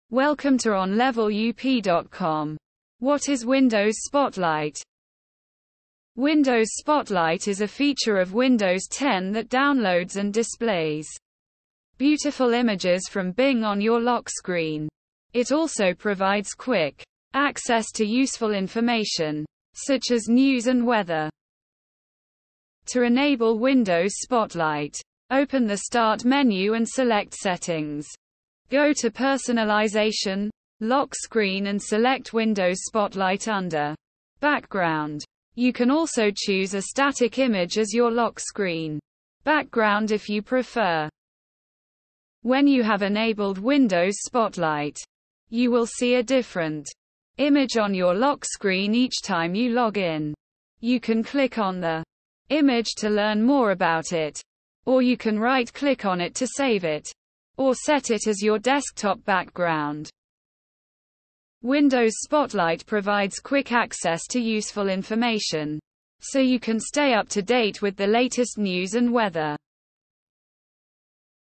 What is Windows Spotlight ?
Windows Spotlight is a feature of Windows 10 that downloads and displays beautiful images from Bing on your lock screen. It also provides quick access to useful information, such as news and weather.